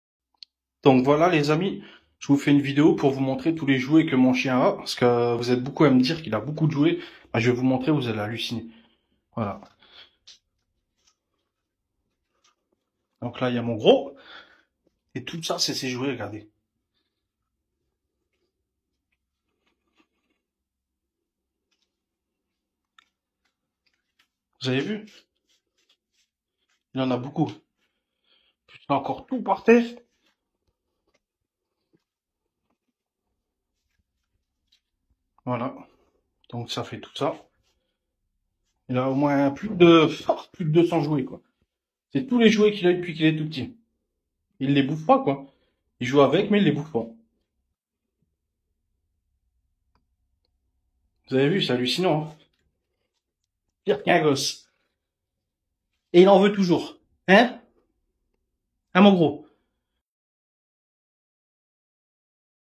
➡️ Kyson Le Boss - Un chien heureux
➡️ Je m'appelle Kyson le Boss, je suis un American Bully, Un amour de Bully

➡️ Production : ACF Productions

Parole de chien : je suis une star